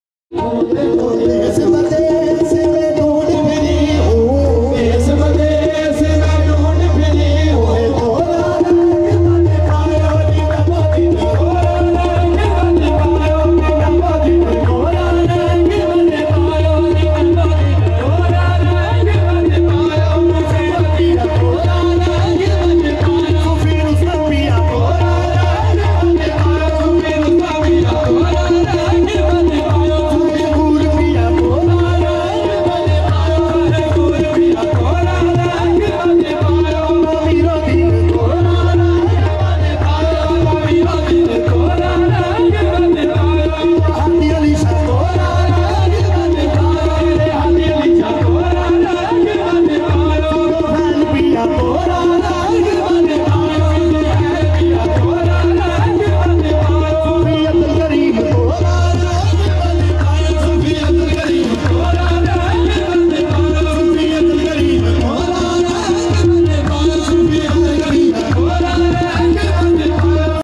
Chishti Mehfil urs Mubarik Sufi Abdul kareem Chishti chak 399
Qari Hassan kaleem chishti
urs Mubarik Sufi Abdul kareem Chishti chak 399

| Darvesh Production | Chishti Mehfil | Latest Kalam 2020 Toba Tek Singh

Toba tek singh Beautiful Qwali Darvesh Production Toba tek singh